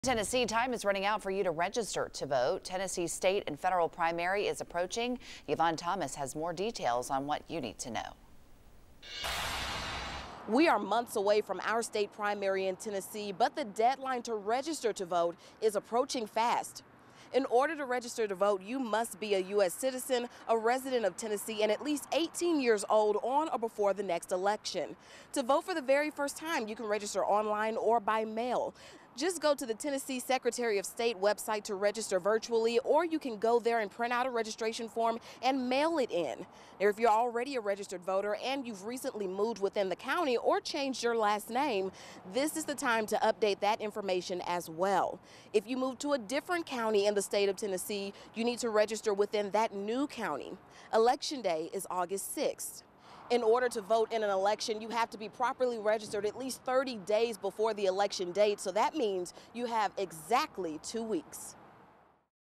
Voter registration deadline two weeks away
Here in Tennessee, time is running out to register to vote. Tennessee's State and Federal primaries are approaching.